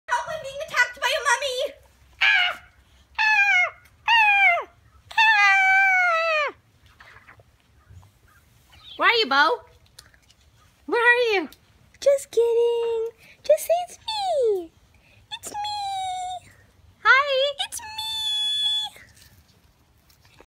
Trying on my Halloween Costume
My sister though that it was  a good idea to dress me up as a mummy, I was not ammused